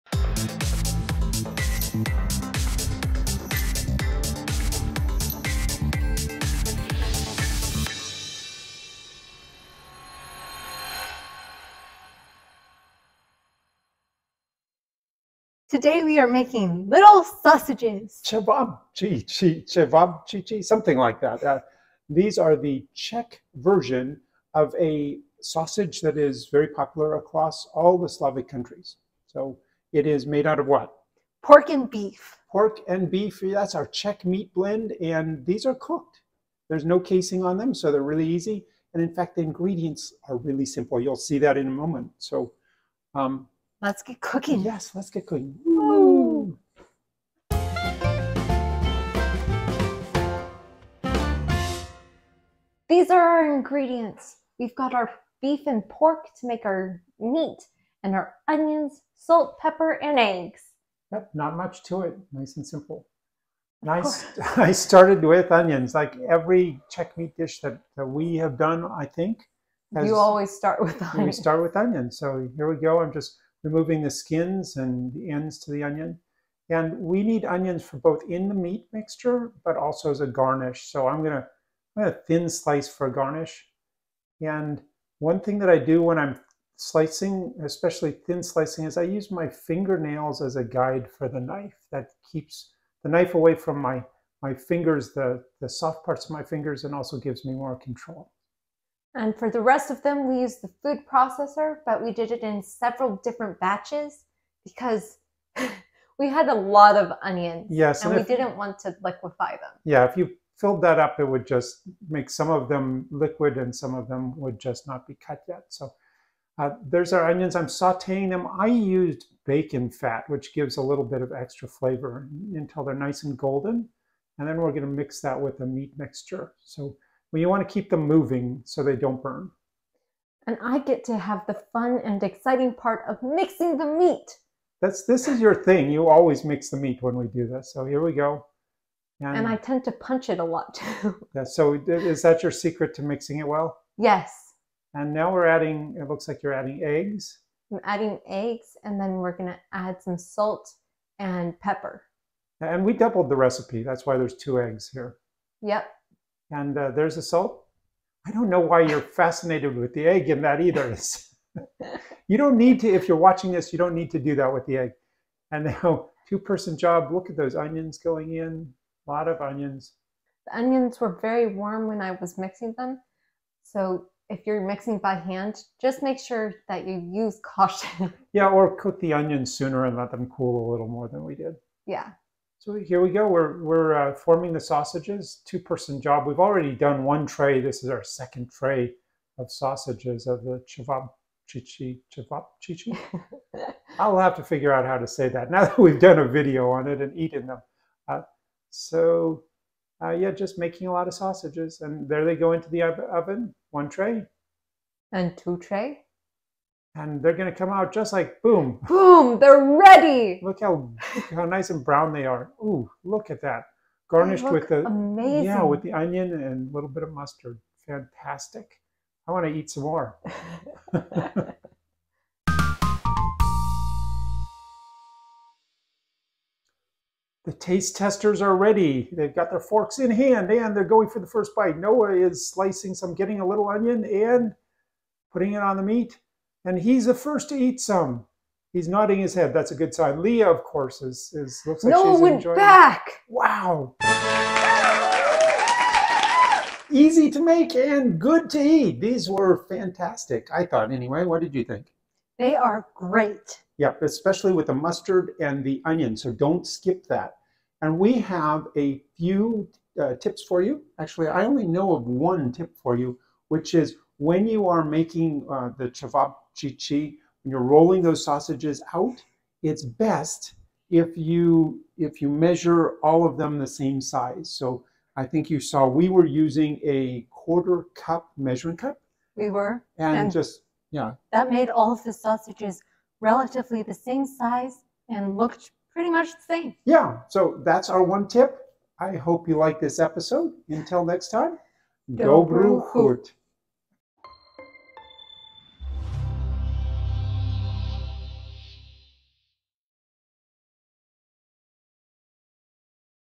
Čevabčiči - Simple Czech Sausages
Čevabčiči (small hand-rolled sausages) are really easy to make and tasty to eat.  Join us on this video as we make this Slavic classic.  You can find the recipe we used here: